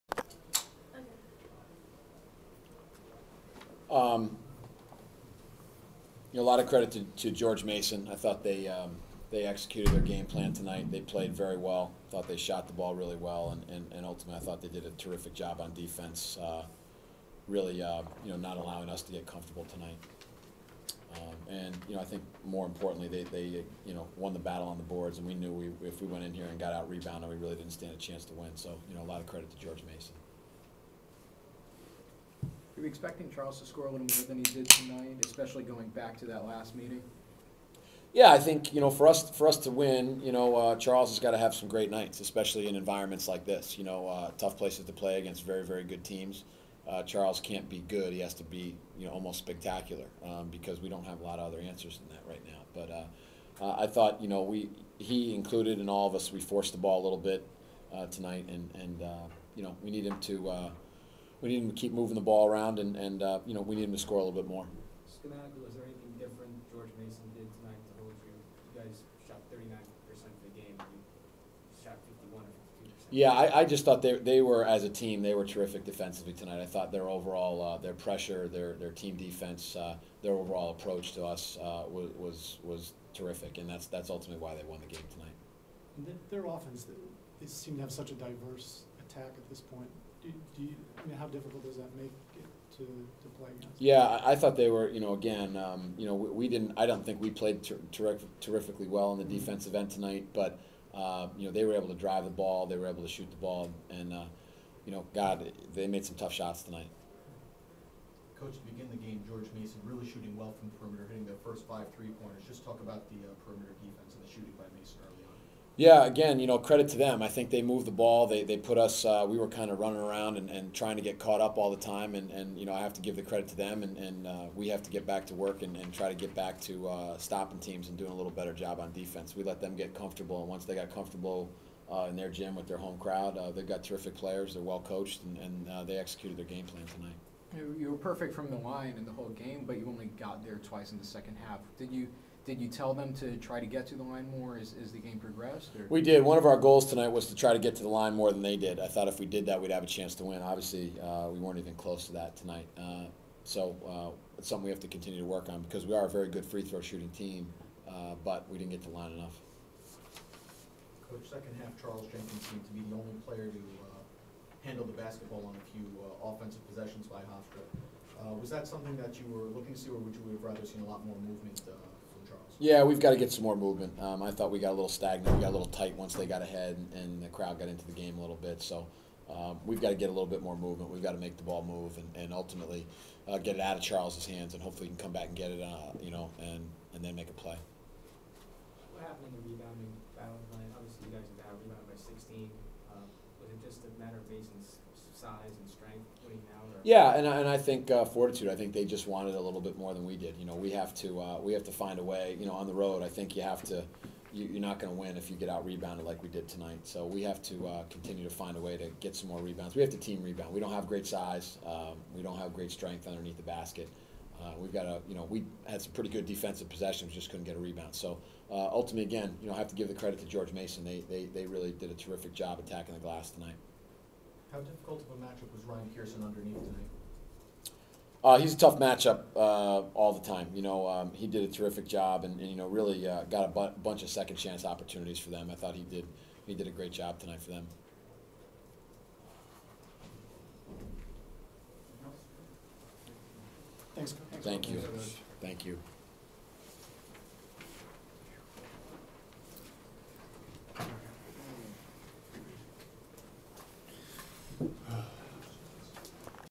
Postgame Hofstra @ George Mason, Mo Cassara
Recorded on February 2, 2011 Hofstra Head Coach Mo Cassara